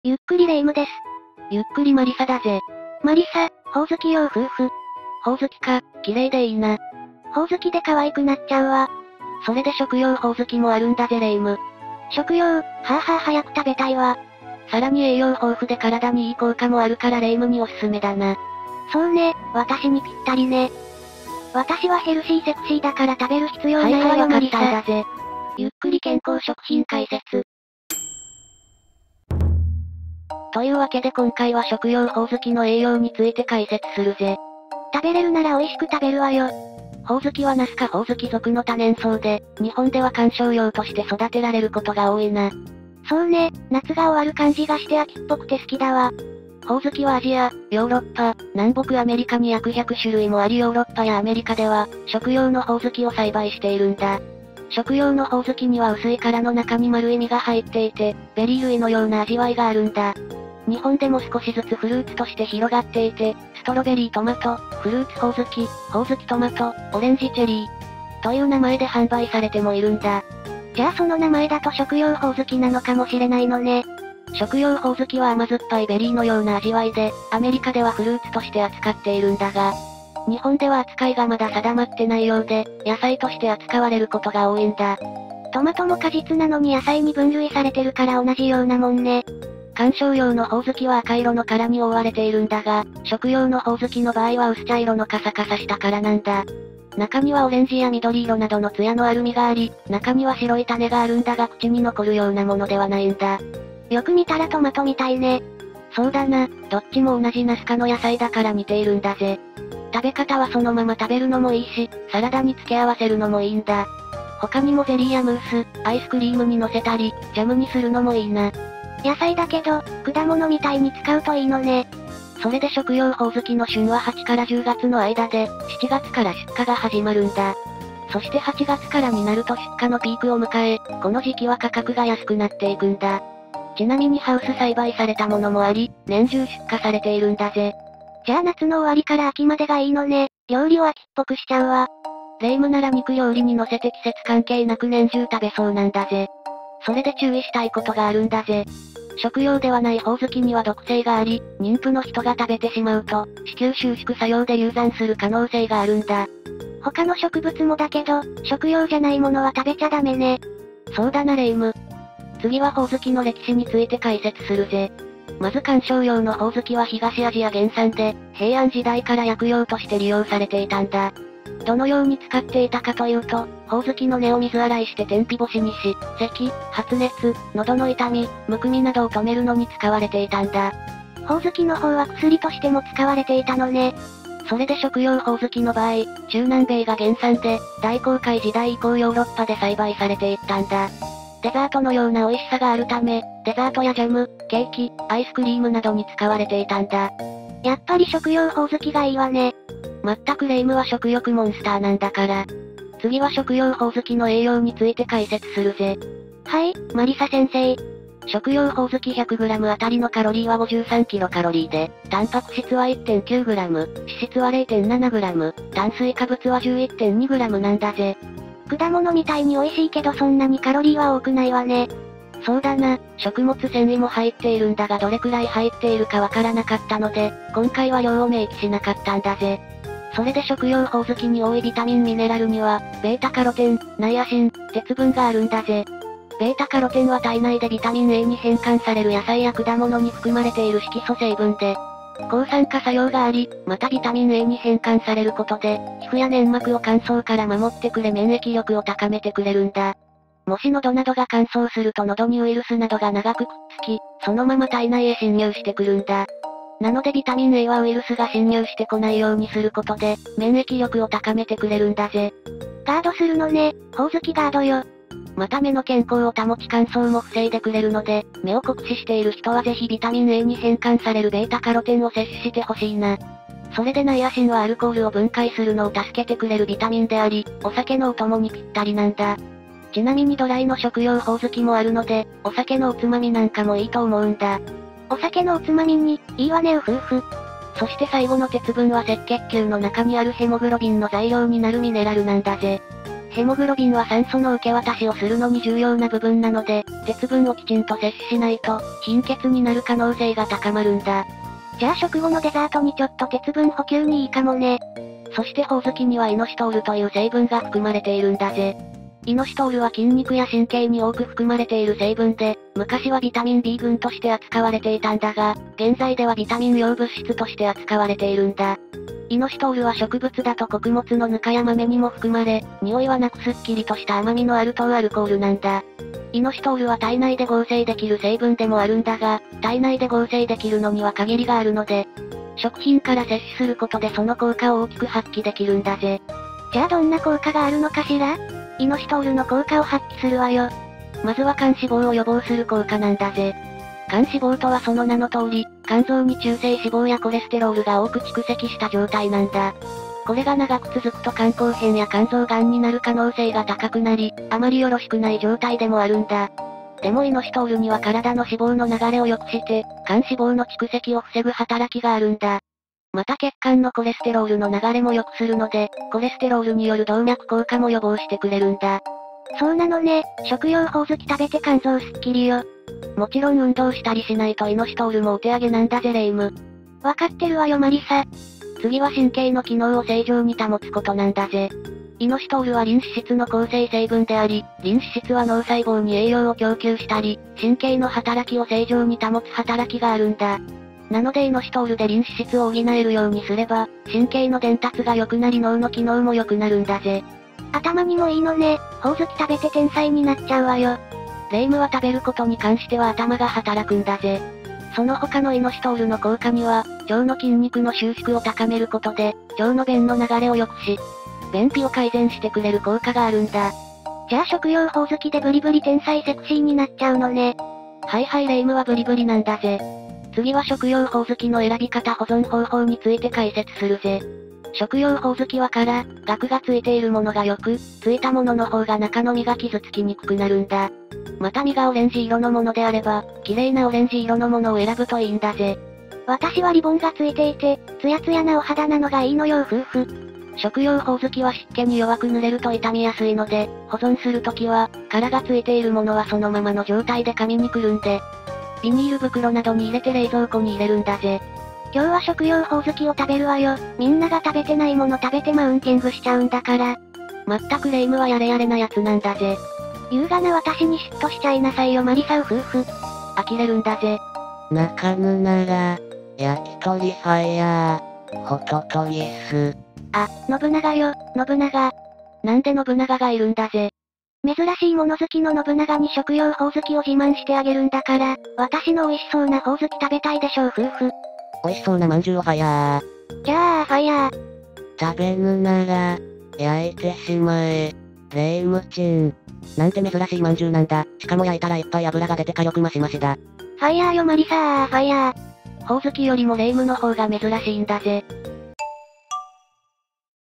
【ゆっくり解説】食用ホオズキの栄養ついて解説！
ホオズキには食べられる種類があることがご存じでしょうか？今回は食べられるホオズキ、食用ホオズキの栄養について解説します。
動画内でも述べていますが、食用ではないホオズキには毒性があるので、商用ではないホオズキは食べないようにしてください！

【目次】
00:00 冒頭
00:37 食用ホオズキについて
02:45 食用ホオズキの歴史
03:28 食用ホオズキの栄養
07:04 食用ホオズキの選び方・保存方法
07:32 レア饅頭

サブチャンネルもゆっくり見ていってね！